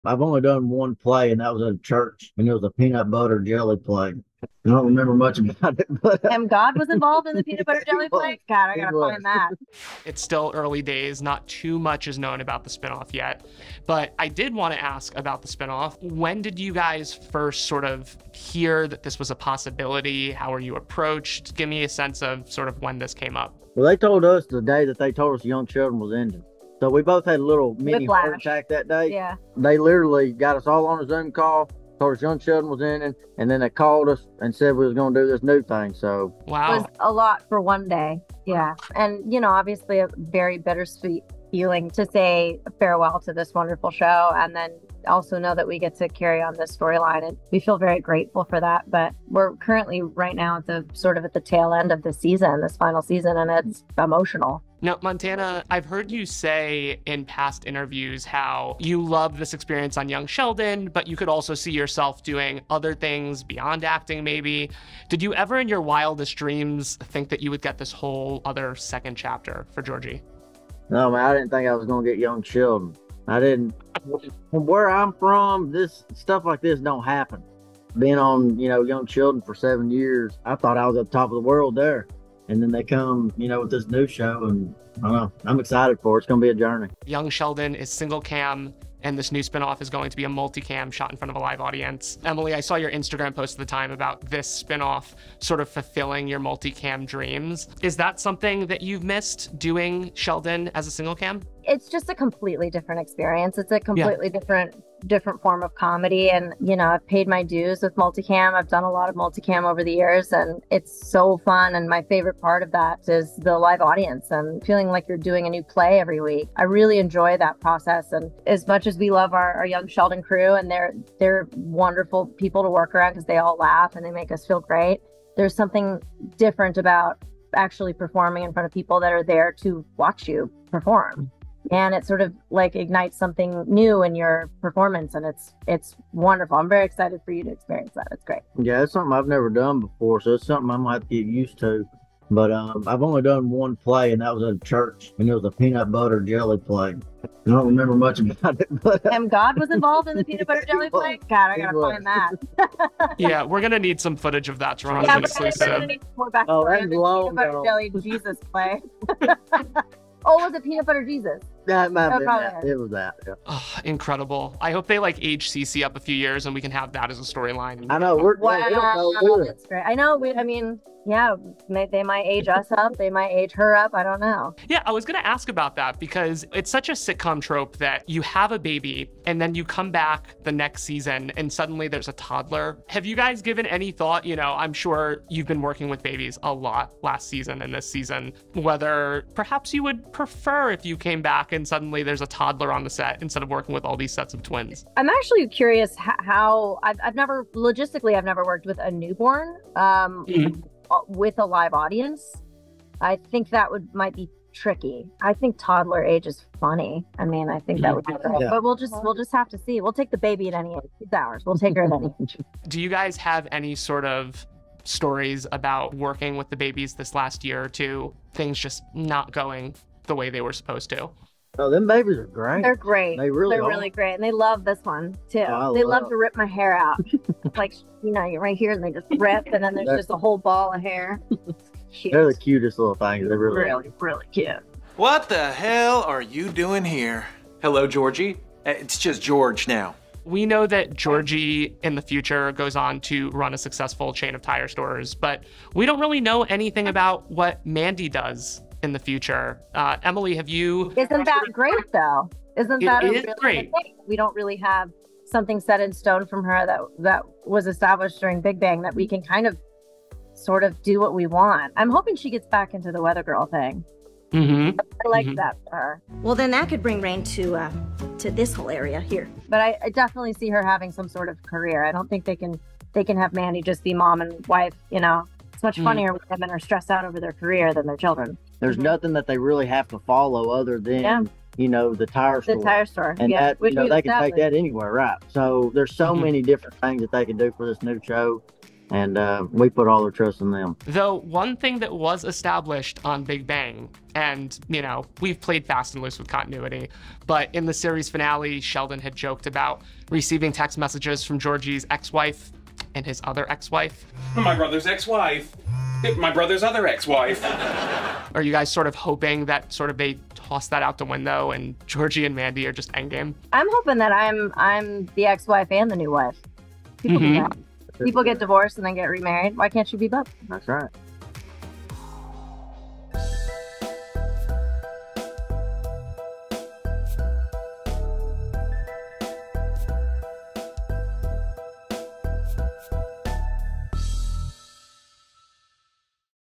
Young Sheldon | Georgie & Mandy Spinoff Cast Interview
In their first interview about their upcoming CBS spinoff, YoungSheldon stars Montana Jordan (Georgie Cooper) and Emily Osment (Mandy McAllister) discuss going from single-camera comedy to multi-cam, and share their feelings on whether the show should retcon BigBangTheory canon and make Georgie and Mandy endgame.